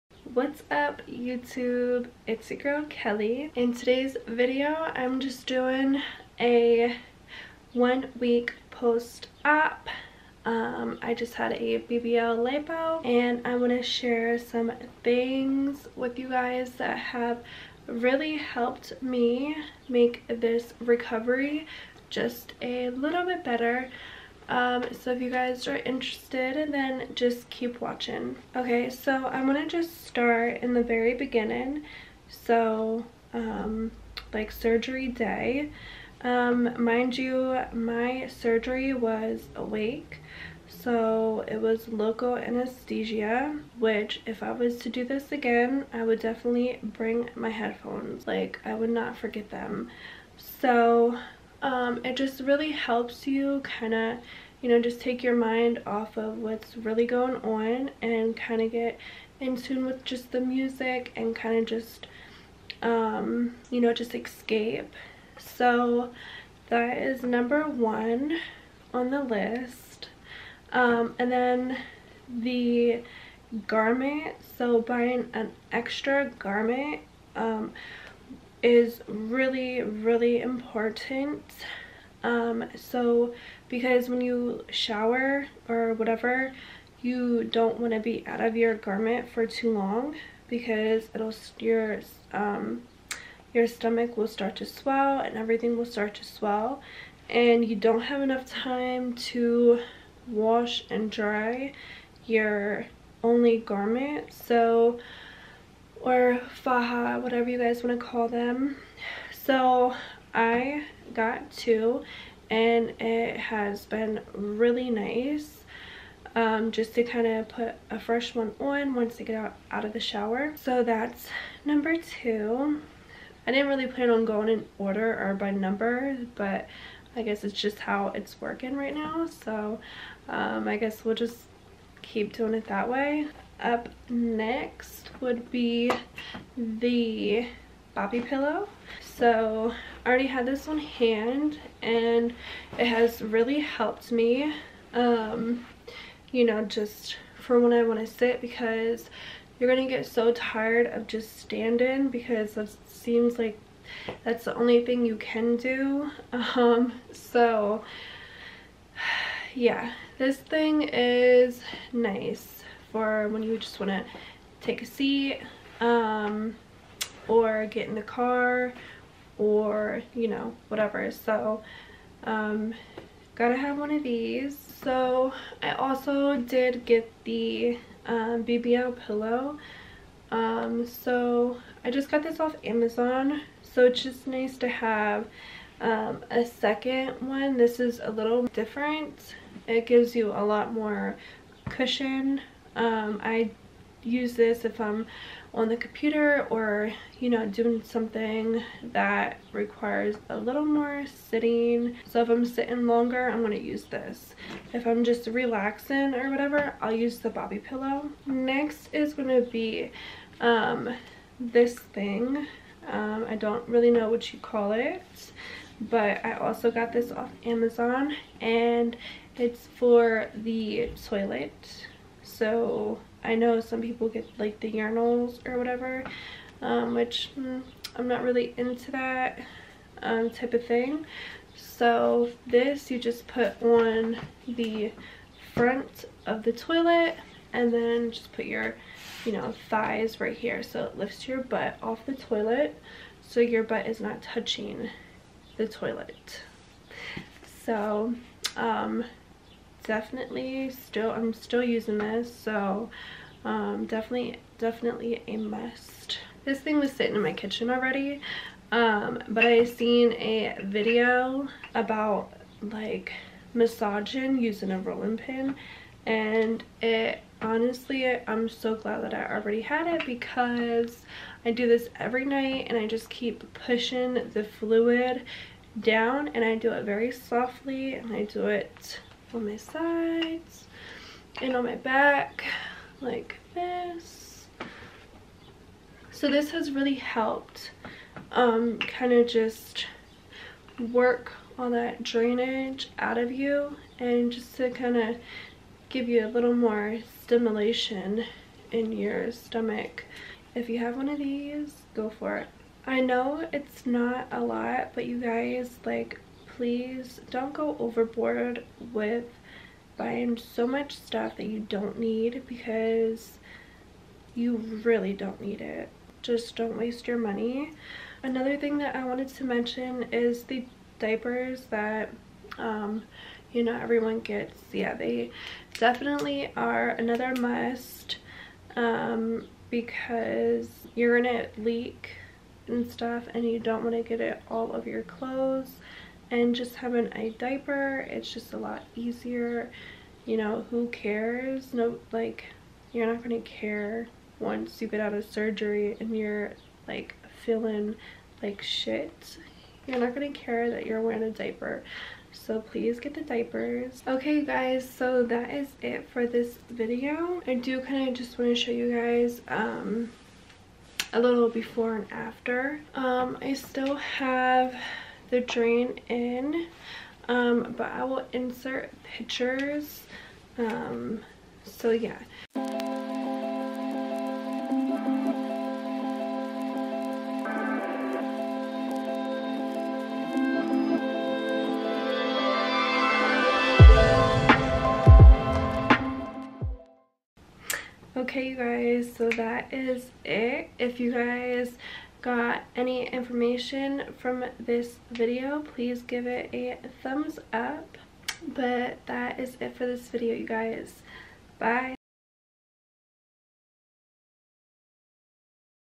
WHAT YOU NEED FOR YOUR BBL/LIPO RECOVERY 💉🍑
A BBL is a HUGE surgery that also can be very high in COST$.. so I get that there are people who feel like they need to accommodate such a big procedure BUT I'm telling you guys, you don't need it ... so SAVE your money where you can because you will need it for more important things like the things I have listed plus the massages you will NEEED.

 ❤️ IG: kelleyclemens_  ...which you could also direct message me if you have more personal questions